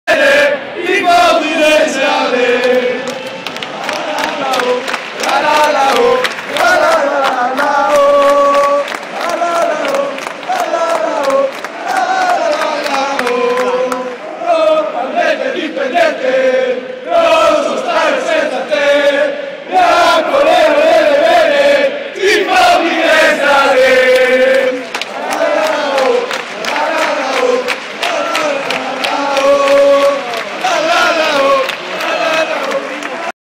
Curva Nord Udine in Udinese Ternana 5-1
Ultras Udinese in Udinese Ternana 5 a 1 Tim Cup.
Totalmente dipendente
Non so stare senza te
Bianconero nelle vene
Tifo udinese aleee
Lallaooo lallaaooo